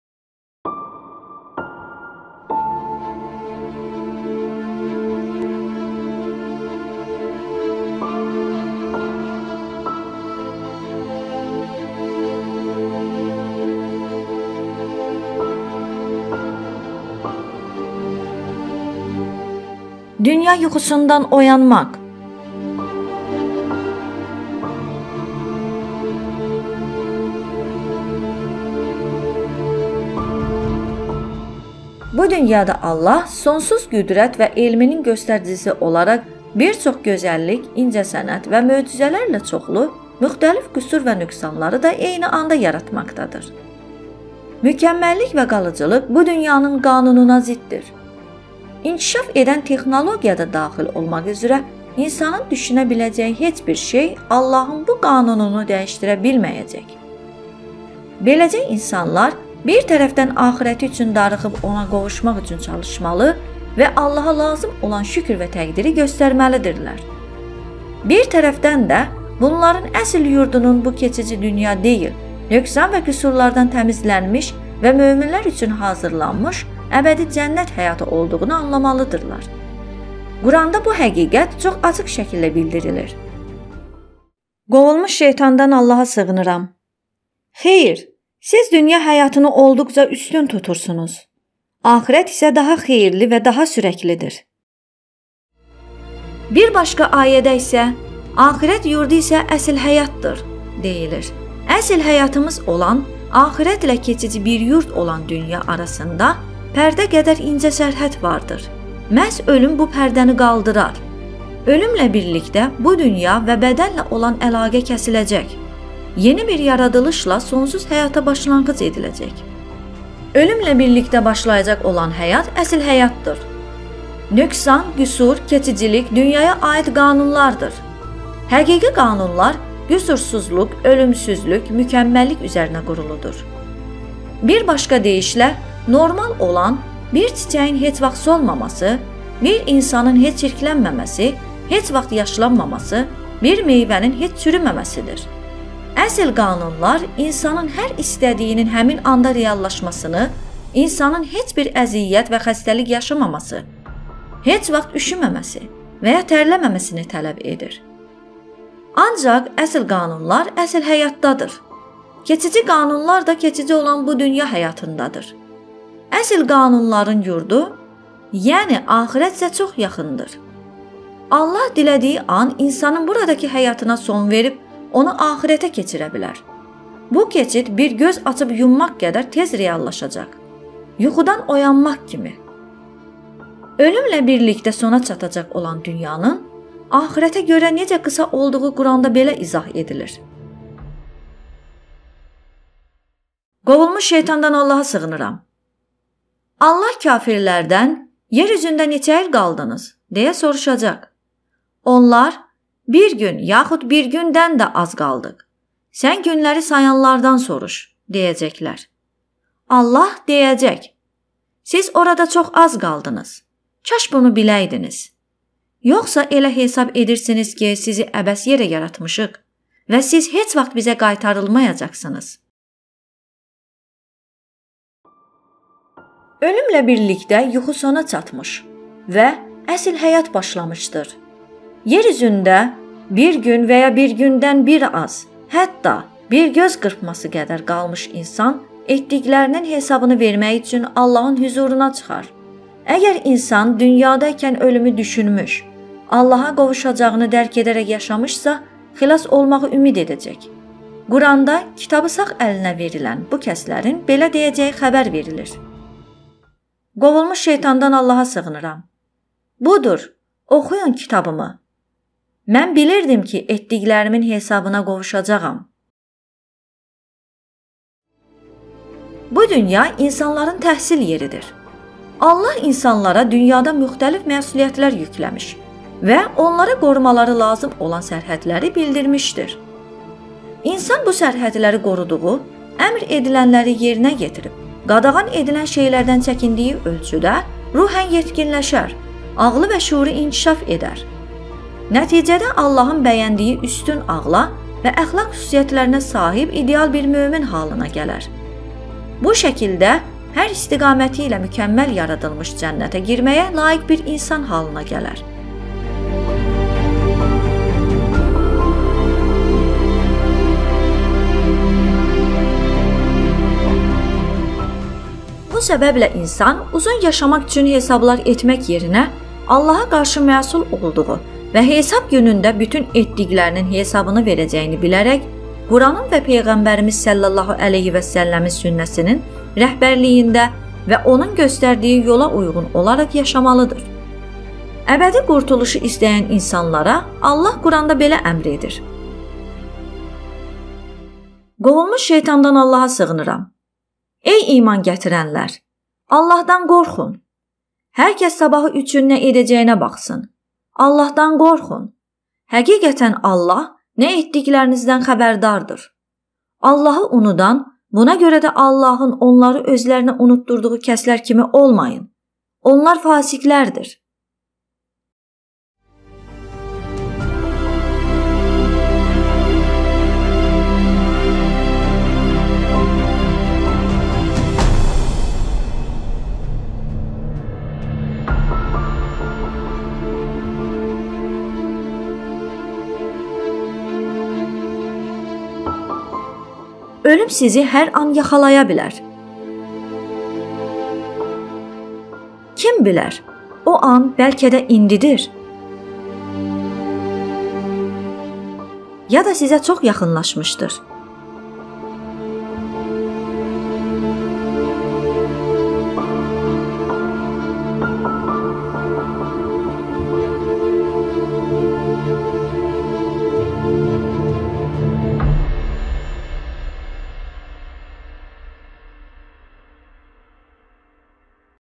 Dünya yuxusundan oyanmaq...
Bu dünyada Allah sonsuz qüdrət və elminin göstəricisi olaraq bir çox gözəllik, incəsənət və möcüzələrlə çoxlu müxtəlif qüsur və nöqsanları da eyni anda yaratmaqdadır. Mükəmməllik və qalıcılıq bu dünyanın qanununa ziddir. İnkişaf edən texnologiya da daxil olmaq üzrə insanın düşünə biləcəyi heç bir şey Allahın bu qanununu dəyişdirə bilməyəcək. Beləcə insanlar bir tərəfdən axirəti üçün darıxıb ona qovuşmaq üçün çalışmalı və Allaha lazım olan şükür və təqdiri göstərməlidirlər. Bir tərəfdən də bunların əsl yurdunun bu keçici dünya deyil, nöqsan və qüsurlardan təmizlənilmiş və möminlər üçün hazırlanmış əbədi cənnət həyatı olduğunu anlamalıdırlar. Quranda bu həqiqət çox açıq şəkildə bildirilir:

Xeyr, siz dünya həyatını olduqca üstün tutursunuz; Axirət isə daha xeyirli və daha sürəklidir. (Əla surəsi, 16-17)

Bir başqa ayədə isə "Axirət yurdu isə əsl həyatdır." (Ənkəbut surəsi, 64) deyilir. "Əsl həyat"ımız olan axirətlə keçici bir yurd olan dünya arasında pərdə qədər incə sərhəd vardır. Məhz ölüm bu pərdəni qaldırar. Ölümlə birlikdə bu dünya və bədənlə olan əlaqə kəsiləcək, yeni bir yaradılışla sonsuz həyata başlanğıc ediləcək.

Ölümlə birlikdə başlayacaq olan həyat əsl həyatdır. Nöqsan, qüsur, keçicilik dünyaya aid qanunlardır. Həqiqi qanunlar; qüsursuzluq, ölümsüzlük, mükəmməllik üzərinə quruludur. Bir başqa deyişlə normal olan, bir çiçəyin heç vaxt solmaması, bir insanın heç çirklənməməsi, heç vaxt yaşlanmaması, bir meyvənin heç çürüməməsidir. Əsl qanunlar, insanın hər istədiyinin həmin anda reallaşmasını, insanın heç bir əziyyət və xəstəlik yaşamaması, heç vaxt üşüməməsi və ya tərləməməsini tələb edir. Ancaq əsl qanunlar əsl həyatdadır; keçici qanunlar da keçici olan bu dünya həyatındadır. Əsl qanunların yurdu, yəni axirət isə çox yaxındır. Allah dilədiyi an insanın buradakı həyatına son verib, onu axirətə keçirə bilər. Bu keçid bir göz açıb-yummaq qədər tez reallaşacaq. Yuxudan oyanmaq kimi... Ölümlə birlikdə sona çatacaq olan dünyanın, axirətə görə necə qısa olduğu Quranda belə izah edilir:

Allah kafirlərdən: “Yer üzündə neçə il qaldınız?” – deyə soruşacaq. Onlar: “Bir gün yaxud bir gündən də az qaldıq. Sən günləri sayanlardan soruş” – deyəcəklər. Allah deyəcək: “Siz orada çox az qaldınız. Kaş bunu biləydiniz! Yoxsa elə hesab edirdiniz ki, sizi əbəs yerə yaratmışıq və siz heç vaxt Bizə qaytarılmayacaqsınız?” (Muminun surəsi, 112-115)

Ölümlə birlikdə yuxu sona çatmış və əsl həyat başlamışdır. Yer üzündə "bir gün və ya bir gündən bir az", hətta "bir göz qırpması" qədər qalmış insan etdiklərinin hesabını vermək üçün Allahın hüzuruna çıxar. Əgər dünyada ikən ölümü düşünmüş, Allaha qovuşacağını dərk edərək yaşamış insanın xilas olmağı ümid edəcək. Quranda “kitabı sağ əlinə verilən” bu kəslərin belə deyəcəyi xəbər verilir:

"...“Budur, oxuyun kitabımı! Mən bilirdim ki, etdiklərimin hesabına qovuşacağam”. (Haqqə surəsi, 19-20)

Bu dünya insanların təhsil yeridir. Allah insanlara dünyada müxtəlif məsuliyyətlər yükləmiş və onlara qorumaları lazım olan sərhədləri bildirmişdir. İnsan bu sərhədləri qoruduğu, əmr edilənləri yerinə yetirib, qadağan edilən şeylərdən çəkindiyi ölçüdə ruhən yetkinləşər, ağlı və şüuru inkişaf edər. Nəticədə Allahın bəyəndiyi üstün ağla və əxlaq xüsusiyyətlərinə sahib ideal bir mömin halına gələr. Bu şəkildə hər istiqaməti ilə mükəmməl yaradılmış cənnətə girməyə layiq bir insan halına gələr.

Bu səbəblə insan, uzun yaşamaq üçün hesablar etmək yerinə Allaha qarşı məsul olduğunu və hesab günündə bütün etdiklərinin hesabını verəcəyini bilərək, Quranın və Peyğəmbərimiz (s.ə.v)-in sünnəsinin rəhbərliyində və onun göstərdiyi yola uyğun olaraq yaşamalıdır. Əbədi qurtuluşu istəyən insanlara Allah Quranda belə əmr edir:

Ey iman gətirənlər! Allahdan qorxun! Hər kəs sabahı üçün nə etdiyinə baxsın. Allahdan qorxun! Həqiqətən, Allah nə etdiklərinizdən xəbərdardır. Allahı unudan, buna görə də Allahın onları özlərinə unutdurduğu kəslər kimi olmayın! Onlar fasiqlərdir. (Həşr surəsi, 18-19)

Ölüm sizi hər an yaxalaya bilər.
Kim bilər o an, bəlkə də indidir, ya da sizə çox yaxınlaşmışdır.